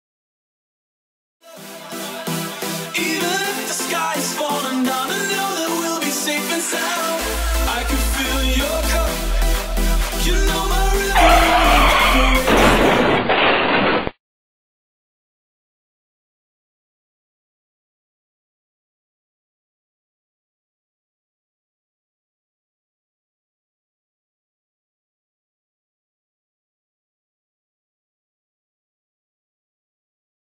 Don't turn into a lastseen.
Drive safely. Don't text.
A road safety awareness campaign by Alfa and Kunhadi.